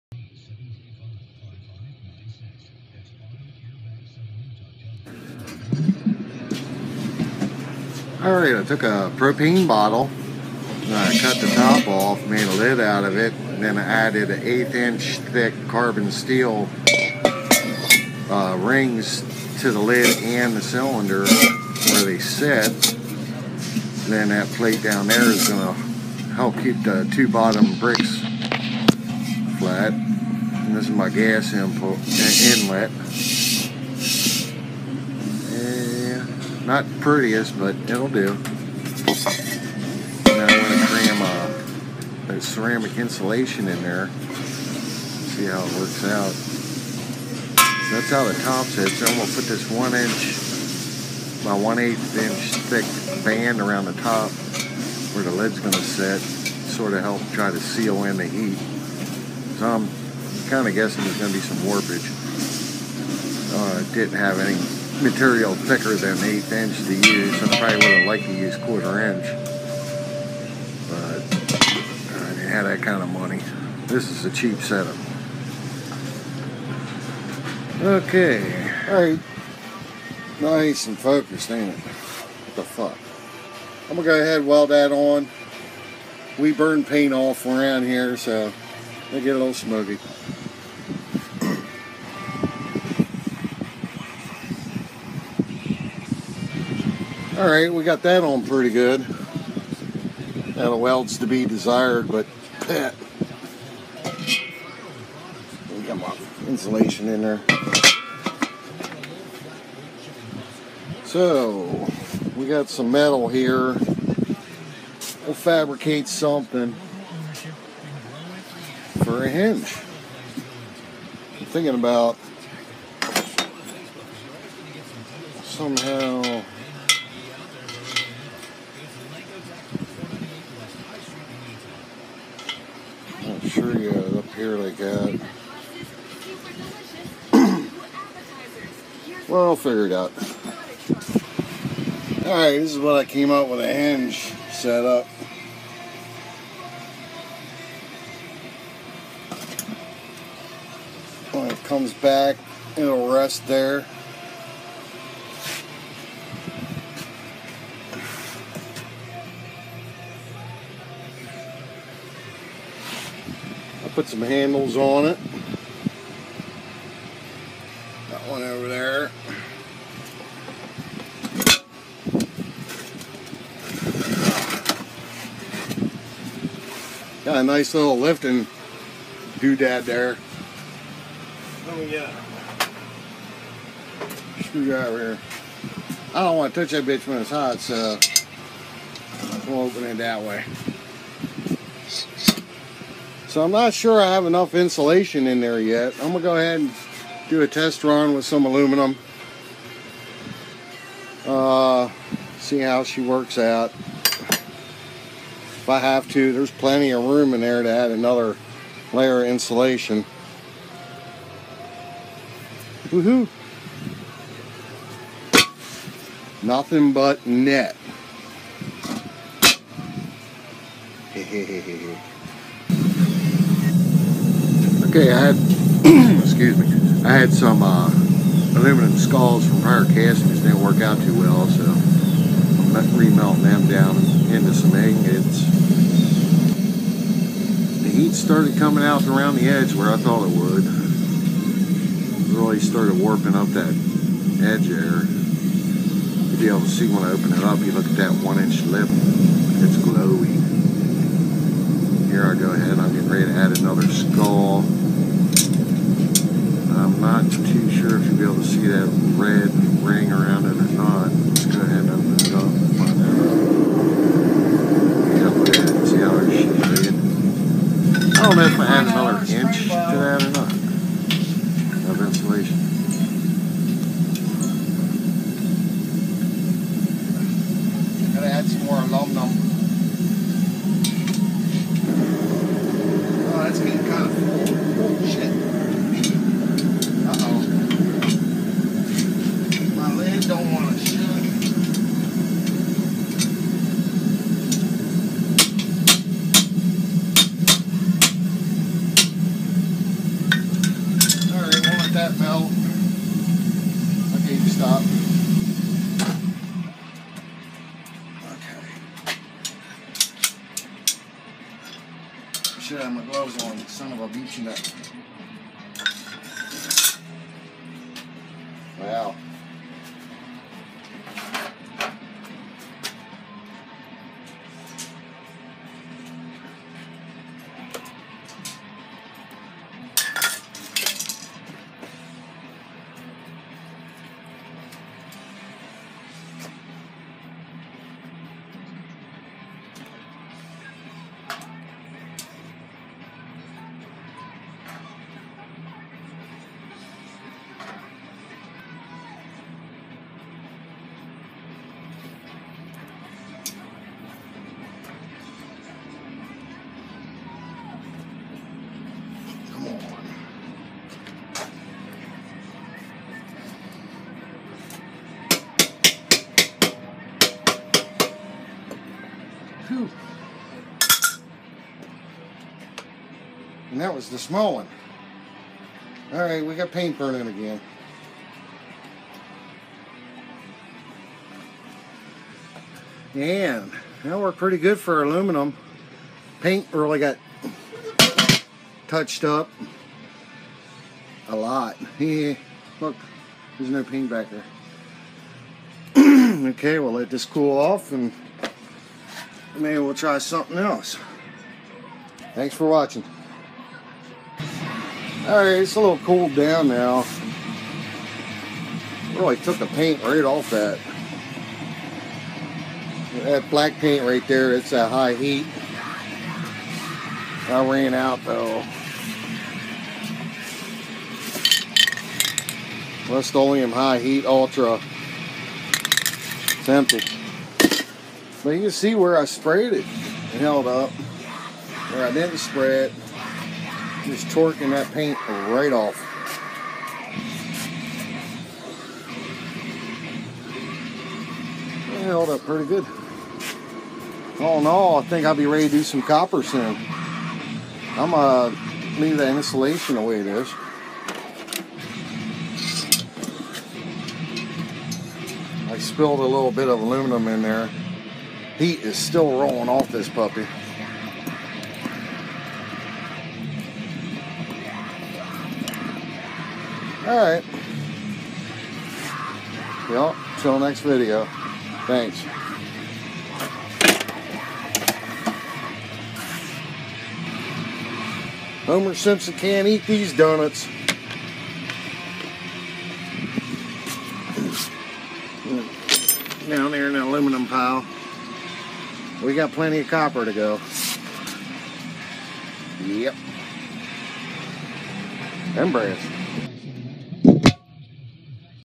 Homemade propane bottle foundry
In all, I spent under $100 for all the material, including the cylinder, it was a fun project, with room to make better, I probably will get a can of high temp paint, and some more ceramic cloth.